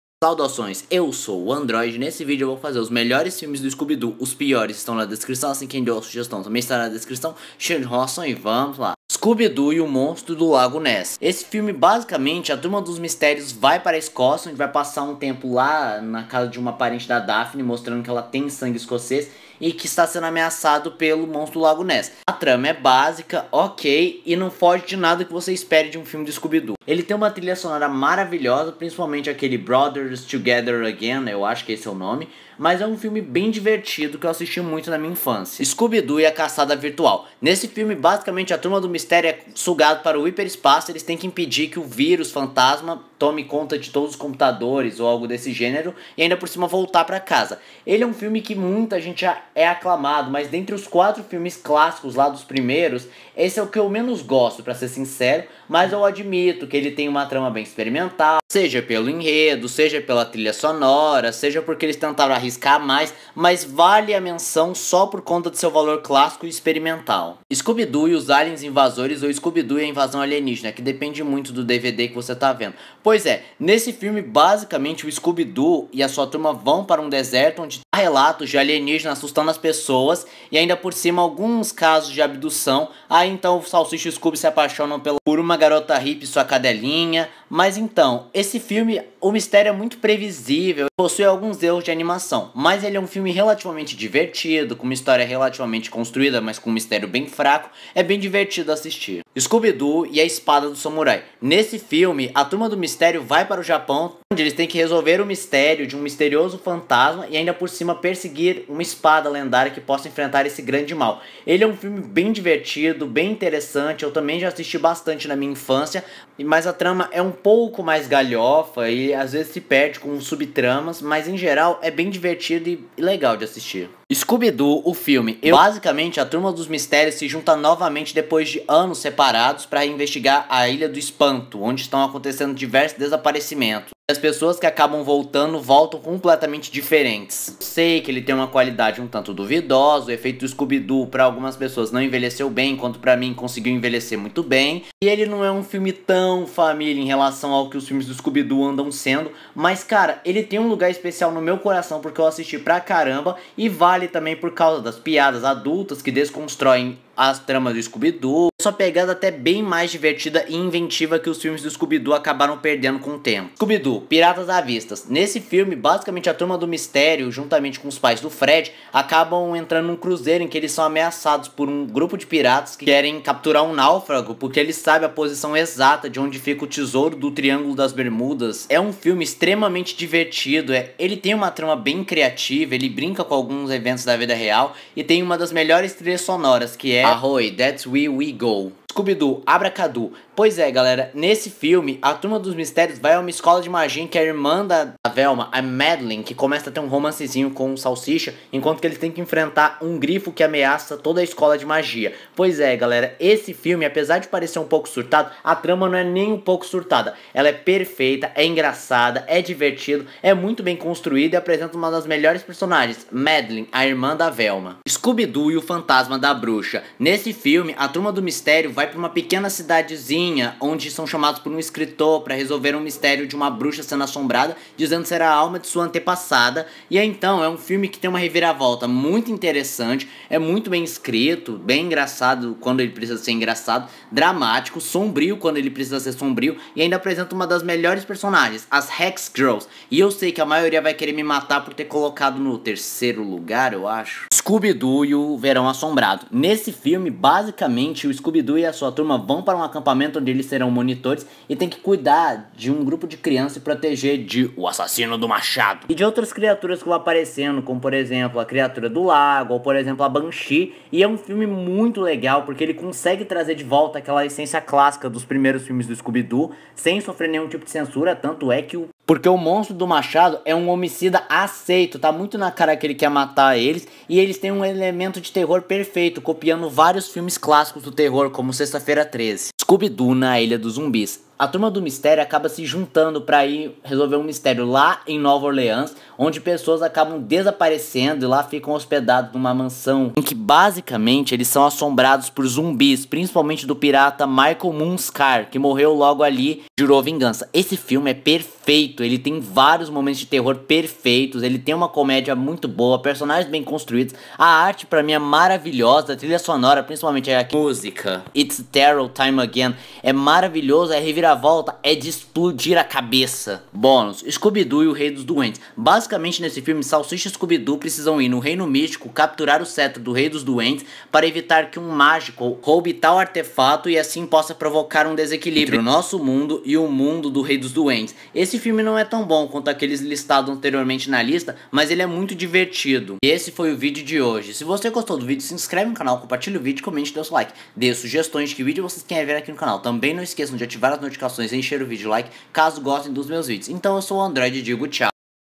Melhores filmes do Scooby Doo
Hoje eu trago os melhores filmes do Scooby Dôo, aqueles que são um clássico, aqueles que mostram como os roteiristas e a própria Warner consegue construir historias boas que vendem.
Se você estiver lendo isso comente: “anão velho” pra eu saber que você leu.